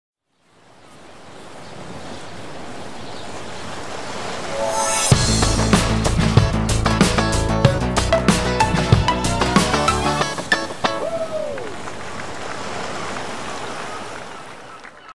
Lacoste Float Film: Episode 2
Lacoste Presents the FloatableOdissey. The First Floatable Sunglasses by Lacoste